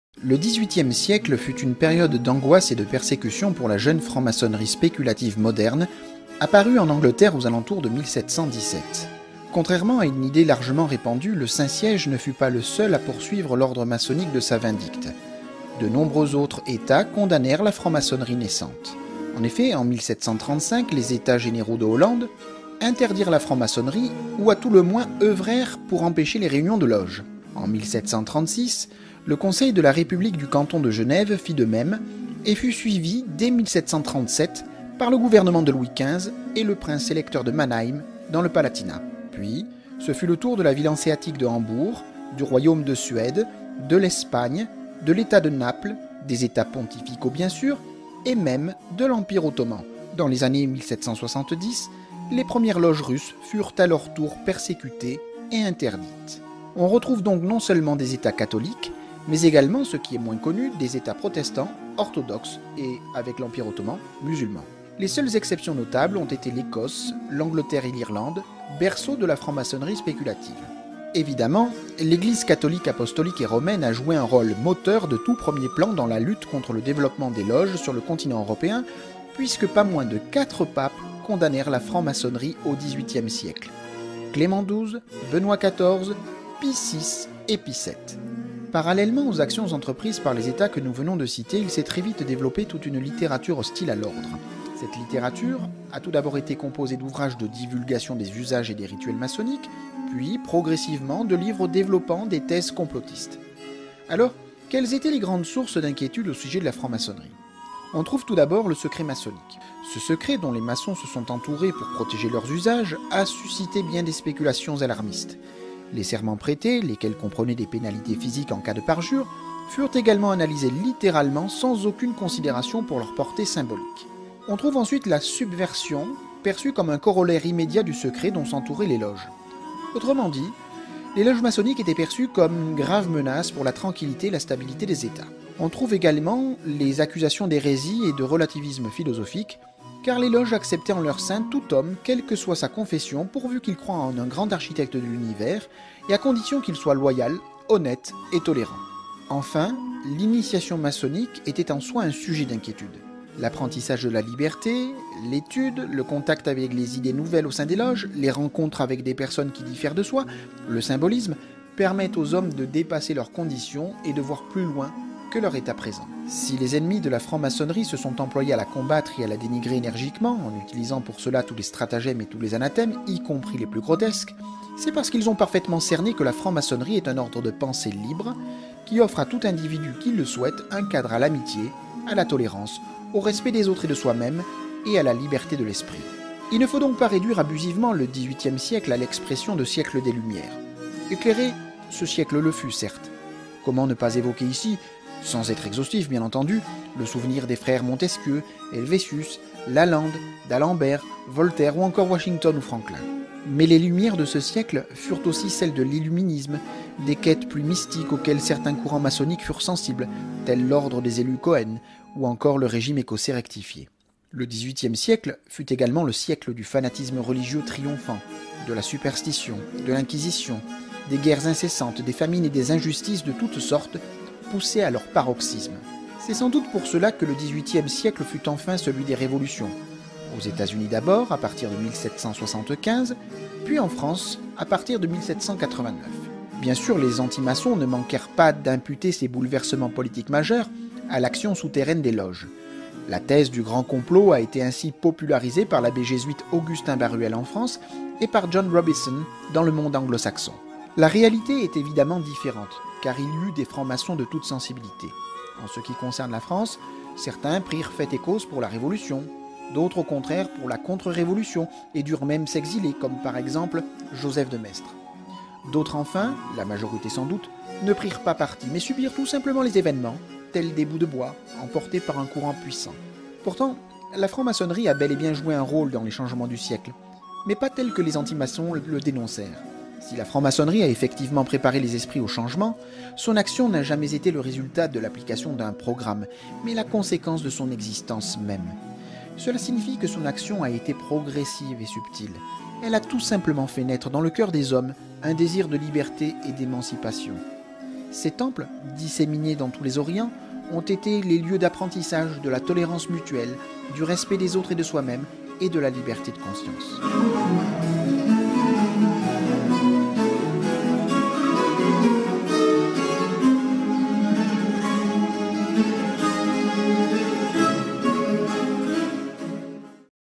L'ANTIMAÇONNISME AU XVIIIe SIECLE
La franc-maçonnerie spéculative a immédiatement été critiquée et combattue par les esprits dogmatiques politiques et religieux dès son apparition en Grande Bretagne aux alentours de 1717 et dans le reste de l'Europe vers 1730.

Vous aimez cette chaîne ? Vous souhaitez contribuer à son développement ? Comment faire ? C'est très simple.